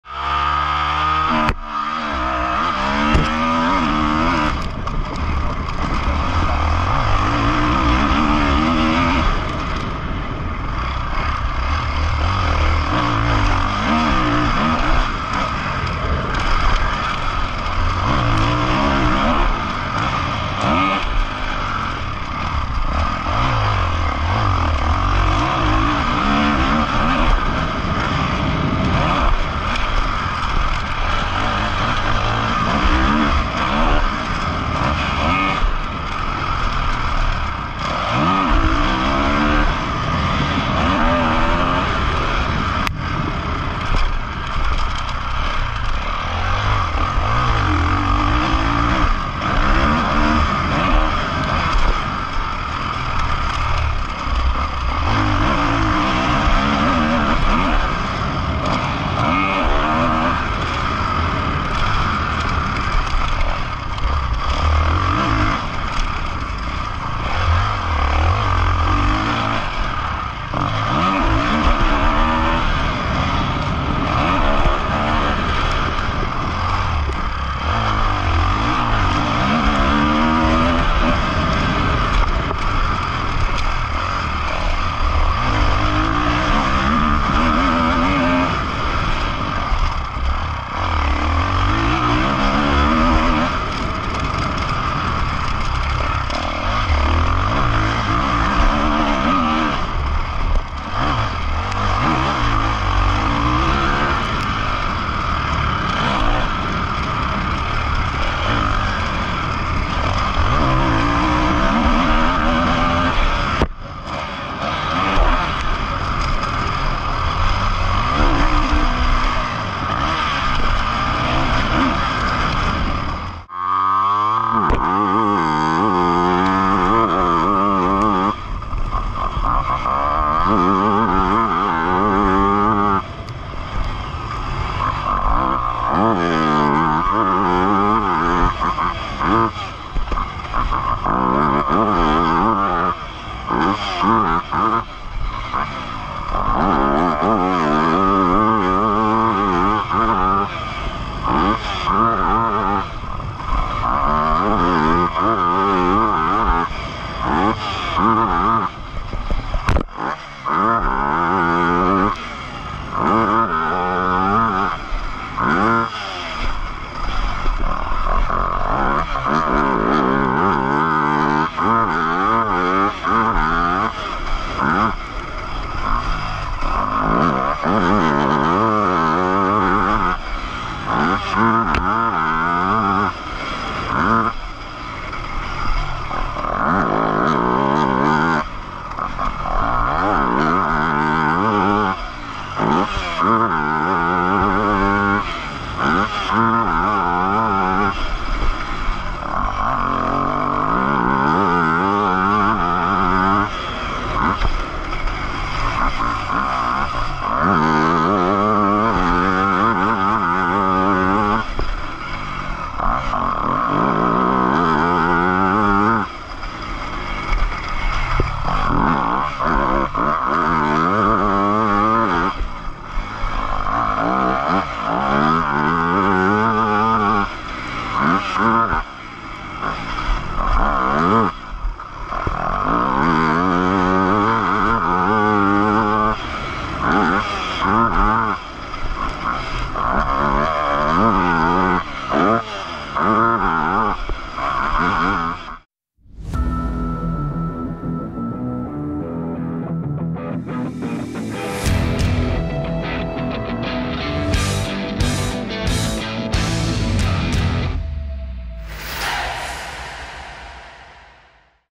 Mantova on board - DUCATI & YAMAHA
On board lap Mantova
Campionato italiano motocross 24MX - Borilli Pro Series 2024

RIDERS
Alessandro Lupino - Ducati Desmo 450
Manuel Iacopi - Yamaha YZF 250